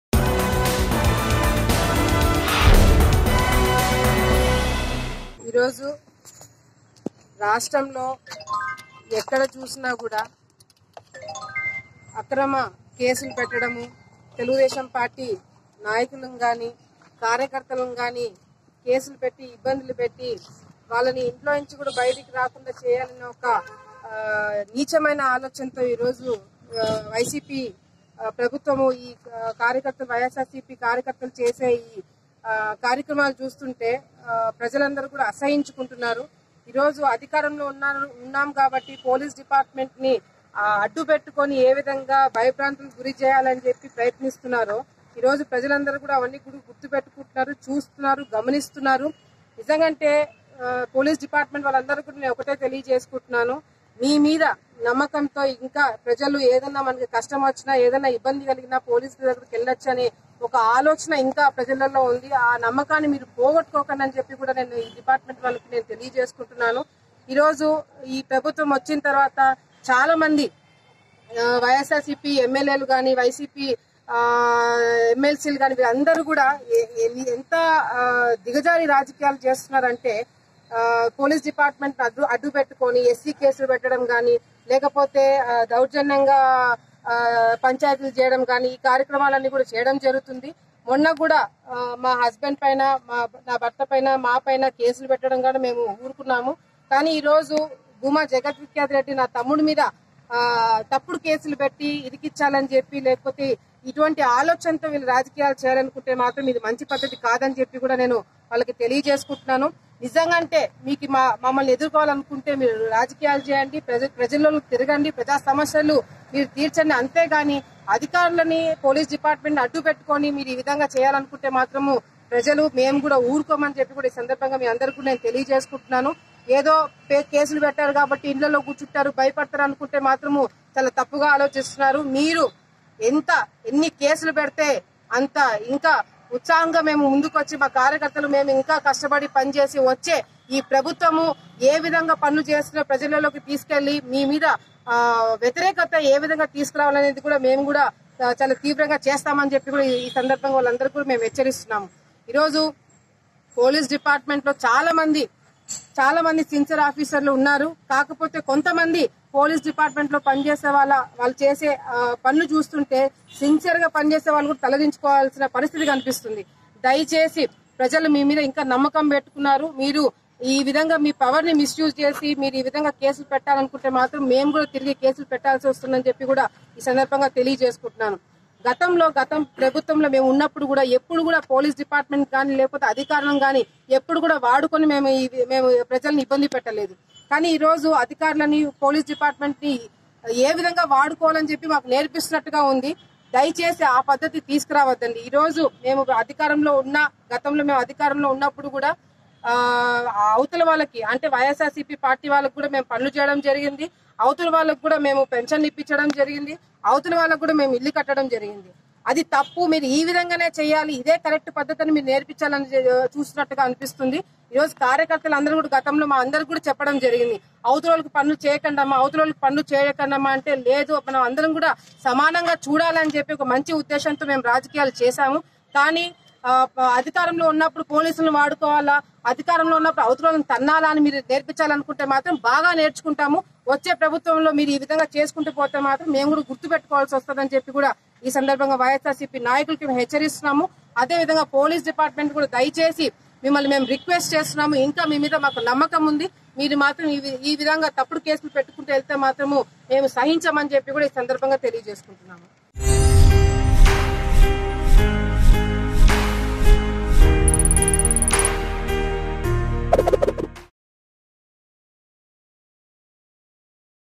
TDP Bhuma Akhila Priya Slams AP Govt Over Filing False Cases On Her Brother | AP News | Mango News
TDP Bhuma Akhila Priya Slams AP Govt Over Filing False Cases On Her Brother.